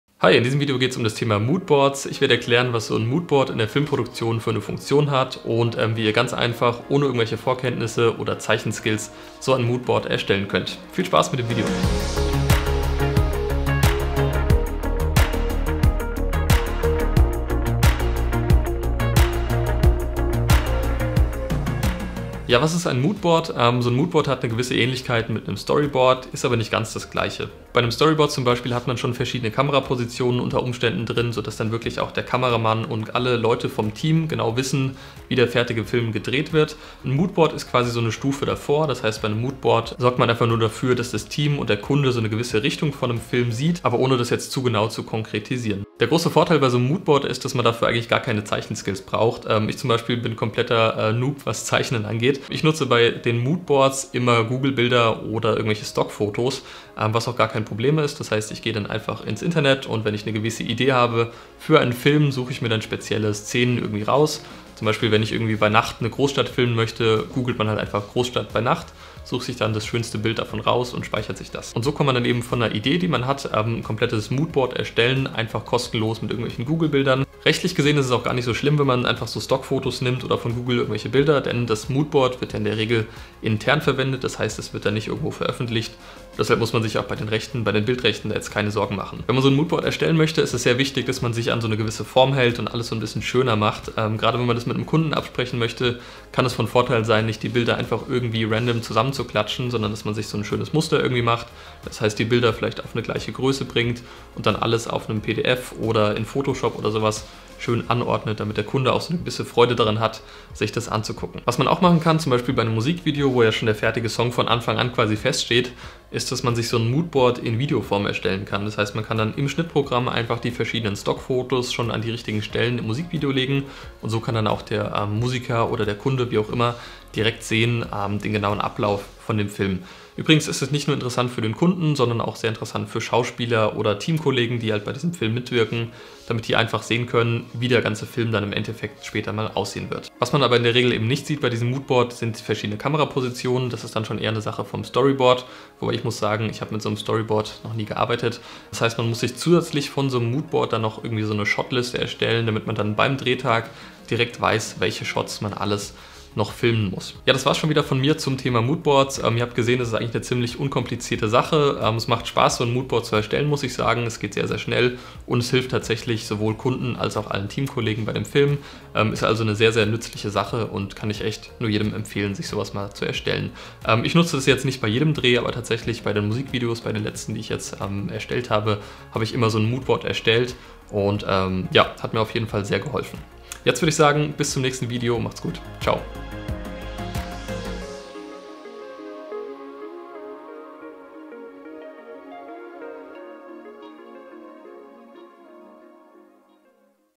Besserer Workflow durch MOODBOARDS - Nutzen beim Dreh & Tipps
In diesem Video geht es um das Thema Moodboards. Warum es bei manchen Drehs sinnvoll ist ein solches Moodboard zu erstellen und wie ihr genau vorgeht, das erfahrt ihr in diesem Video. Viel Spaß! :)

➤ *Musik aus diesem Video (erhalte zwei Monate gratis, wenn du dich über meinen Link bei Artlist registrierst)